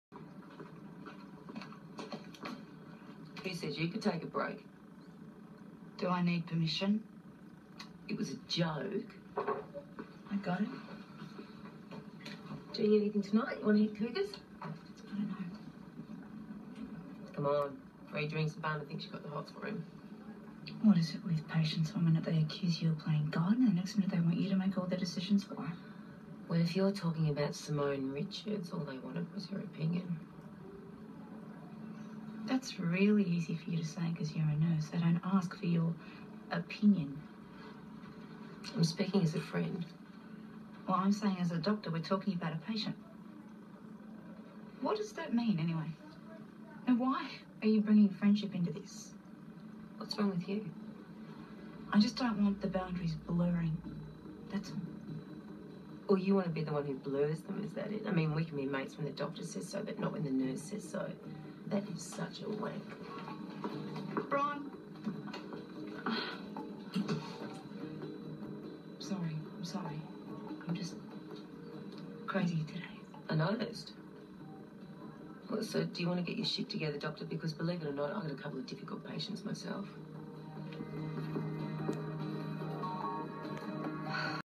Bron and Charlotte fall out ( Libby Tanner )
Charlotte takes her bad mood out on Bron